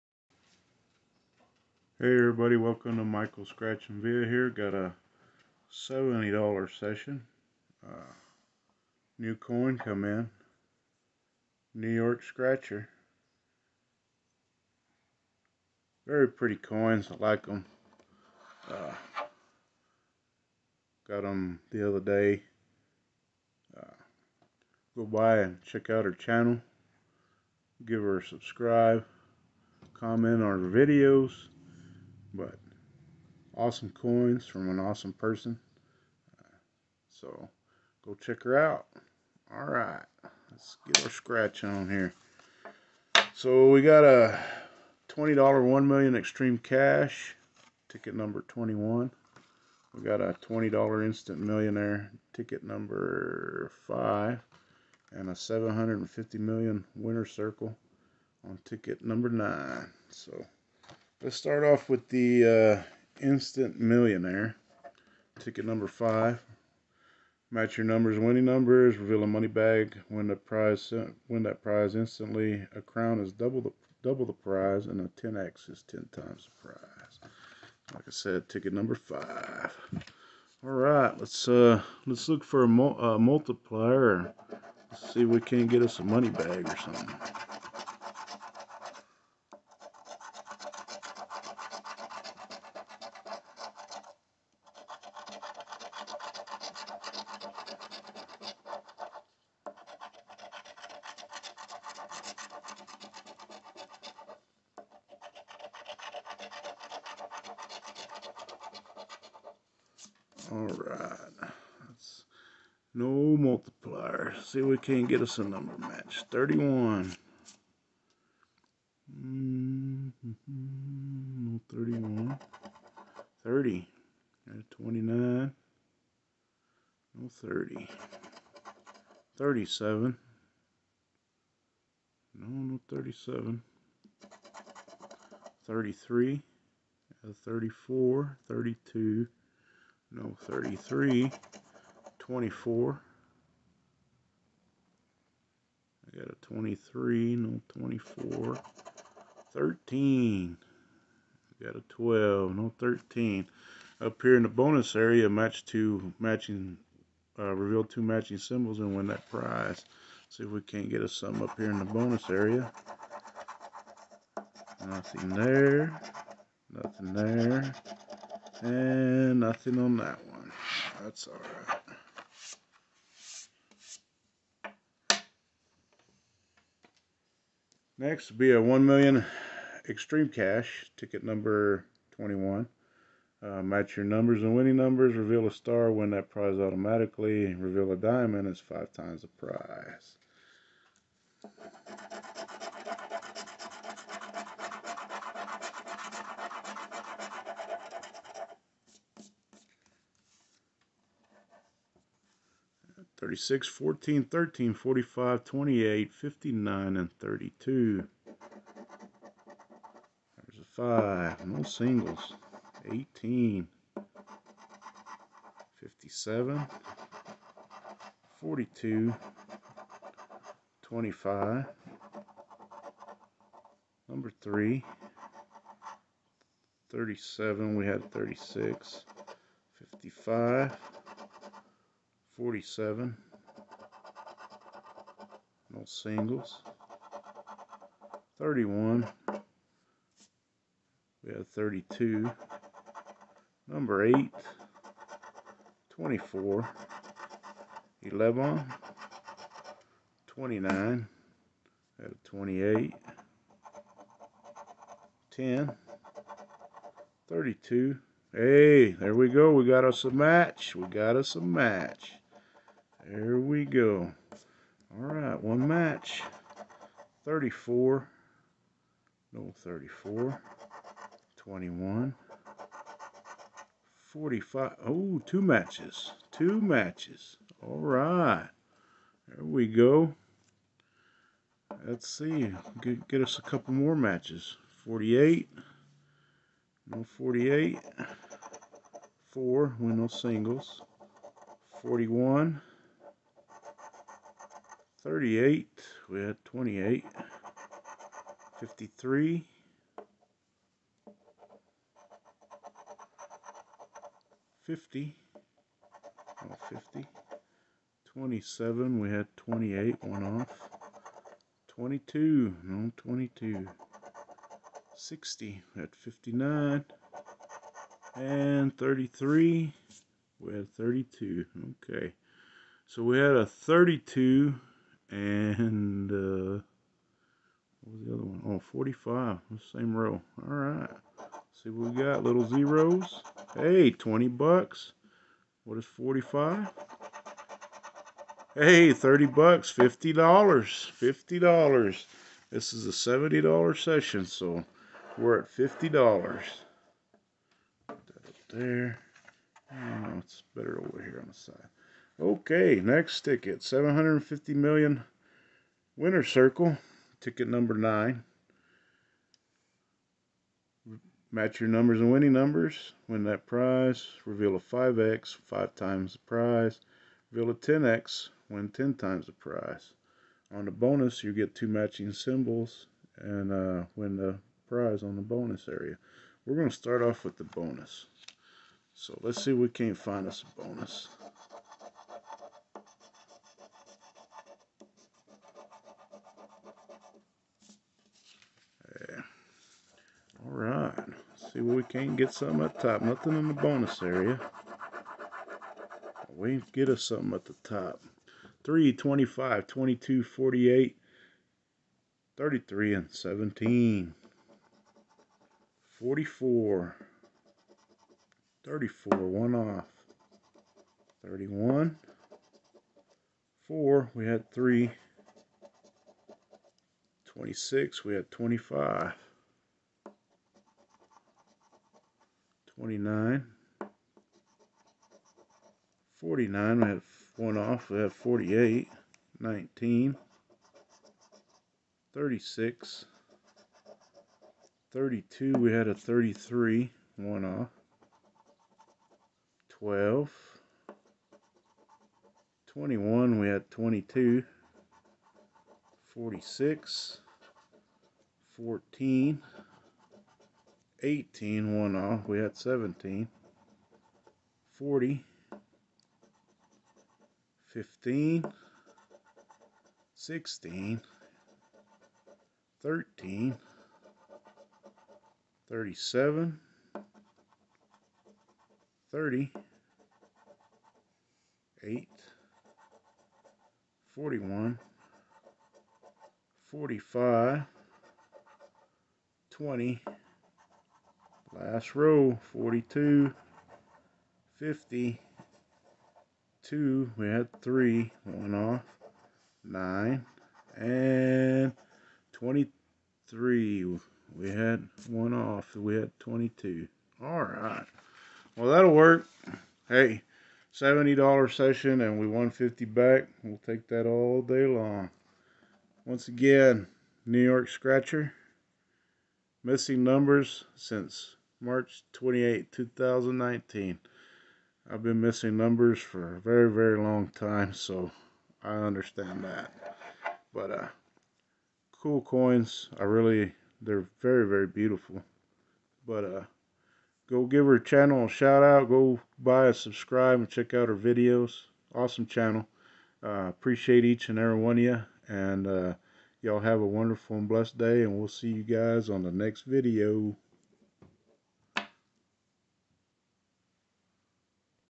Texas Lottery New coin Alert New York Scratcher. 🍀🍀💜💜🤠
New York Scratcher💜
$70 DOLLAR SESSION🍀🍀🍀💰💰💰🤠
1 $20 INSTANT MILLIONAIRE
1 $20 $1,000,000 EXTREME Cash
1 $30 $750 MILLION Winner's Circle

Remember to gamble responsibly, set a budget and stick to it.